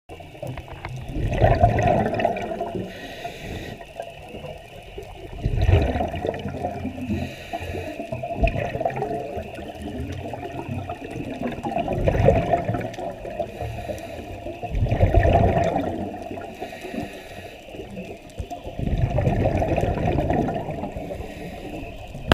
Marylins Cut Horse-eye Jacks
A swim by of about 7-8 Horse eye jacks on top of the wall at Marylins cut- Little Cayman island on 22May2016